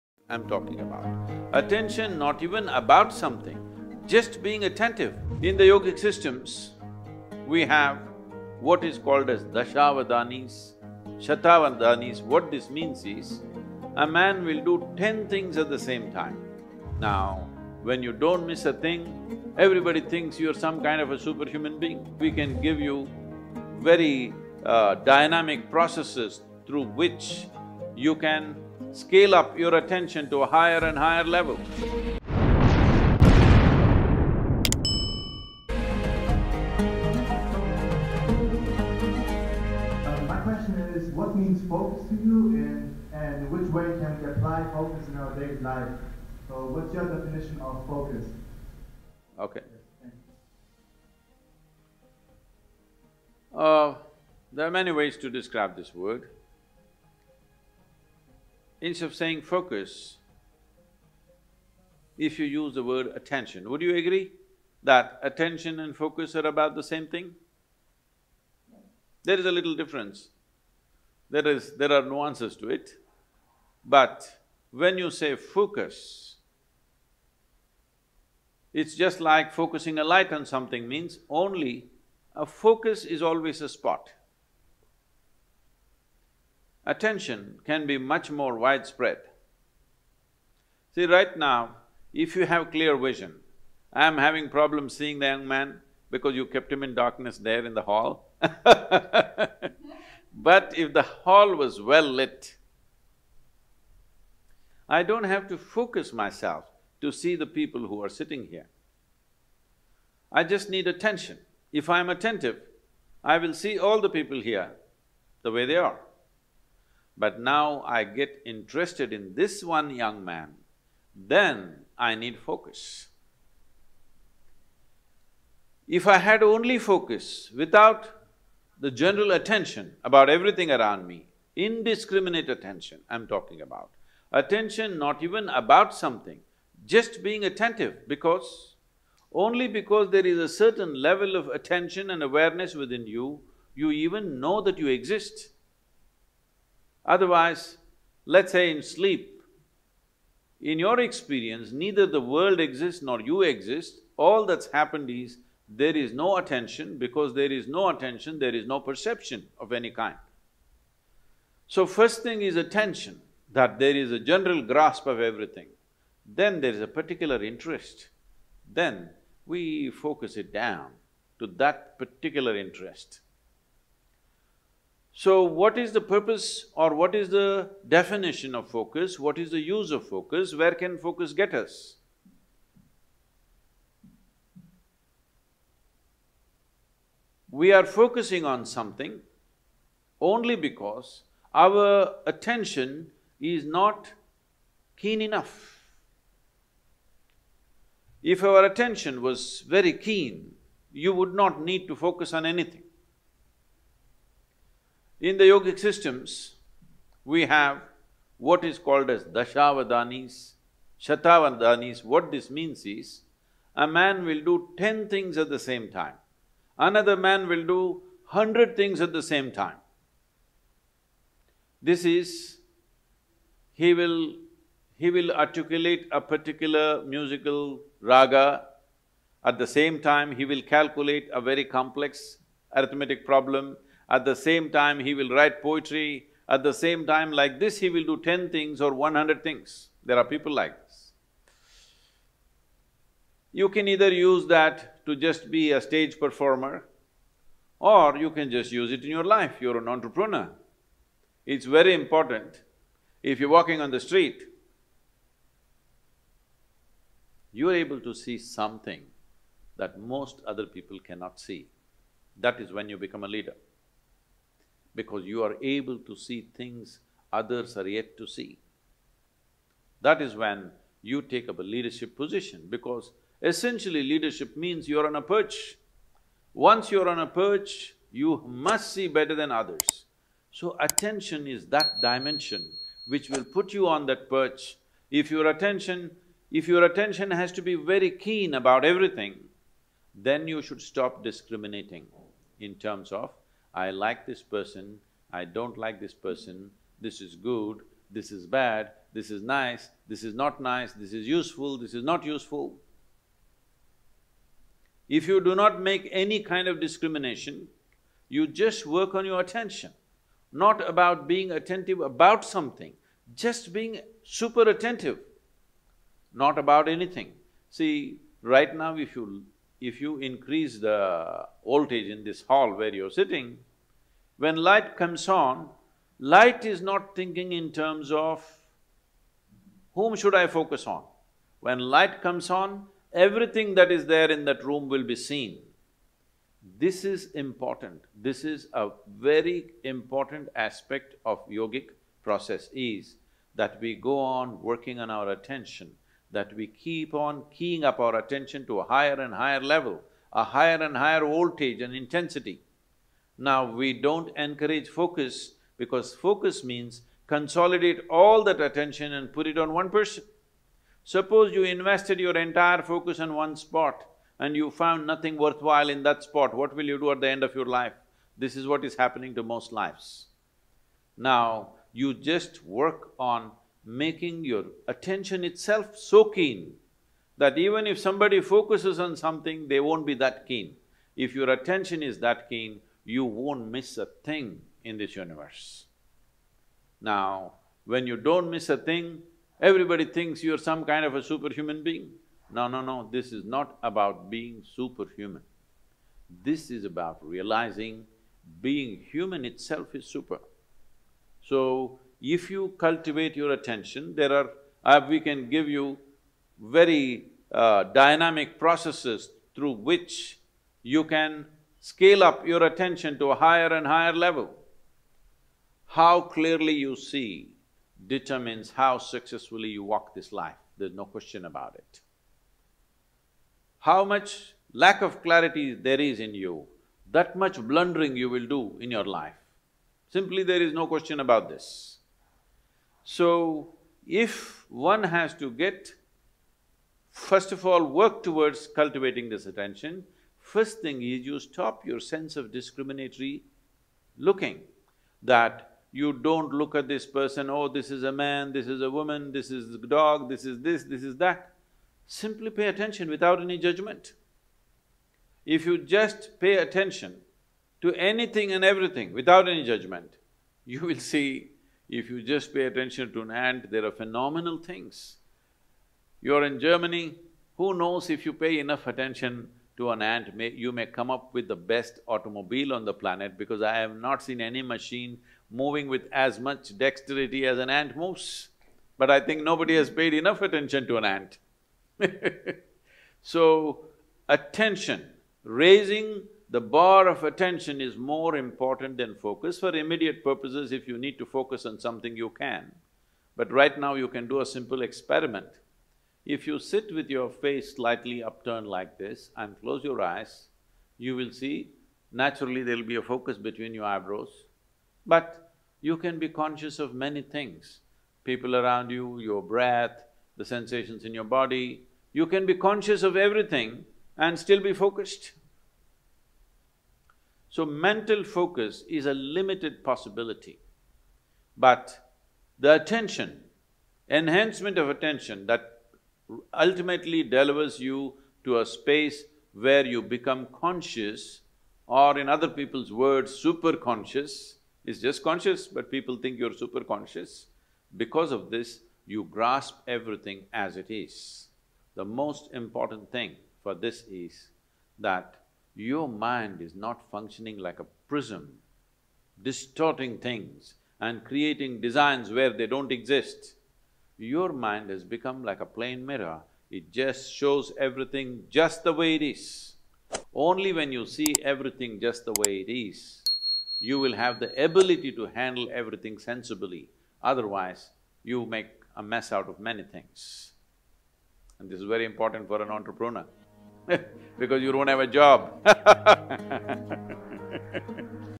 Best Way to Enhance Your Focus | Sadhguru
Uncover the 'Best Way to Enhance Your Focus' in this enlightening video with Sadhguru. Gain valuable insights into improving concentration and mental clarity from Sadhguru's profound wisdom. This video offers practical tips and techniques to sharpen your focus, enhance cognitive abilities, and maintain a steady mind amidst life's distractions. Ideal for students, professionals, and anyone looking to boost their focus and productivity in both personal and professional spheres.

This video is a creative commons from the channel " Sadhguru"